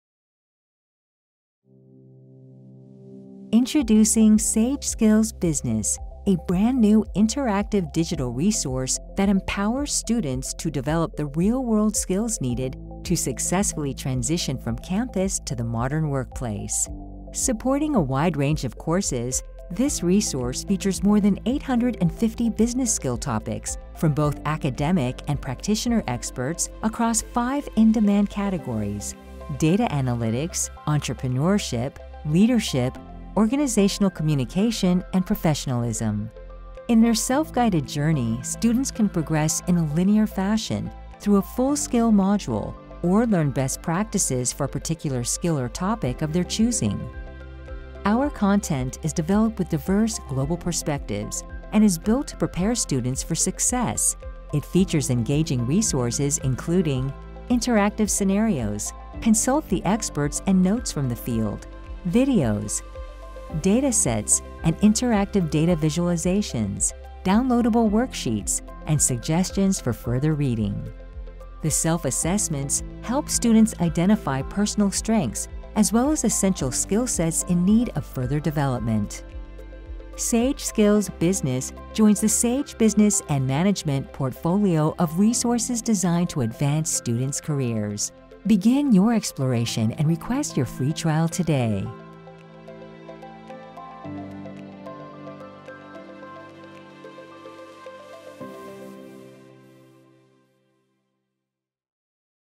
SAGE Skills: Business Trailer
SAGE Skills: Business empowers students to develop and practice real-world skills needed to successfully transition from campus to the modern workplace through interactive self-assessment, virtual scenarios, downloadable data, and expert insights. Supporting a wide range of courses, this resource offers students a self-guided journey to gain the career-ready skill sets essential for professional success.